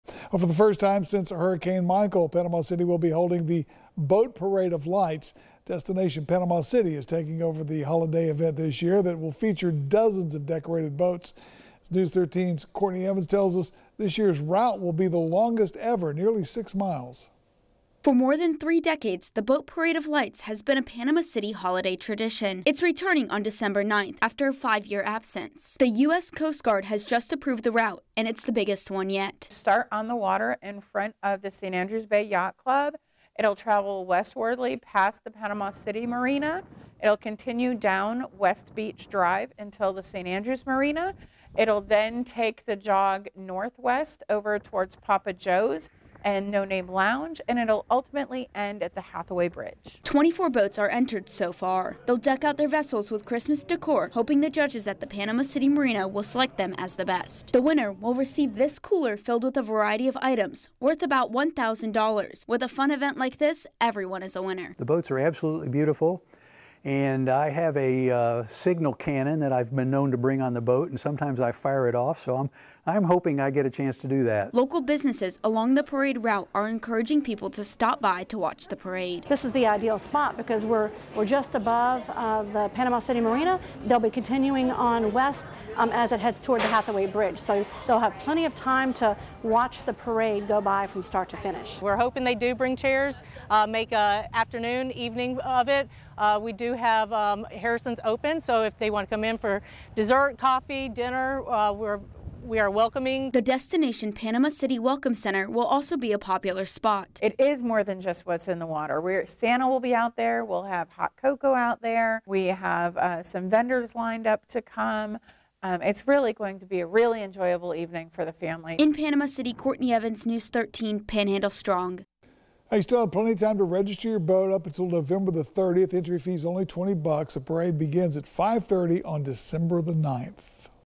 Boat Parade of Lights returns to Panama City
Panama City will hold the Boat Parade of Lights for the first time since Hurricane Michael.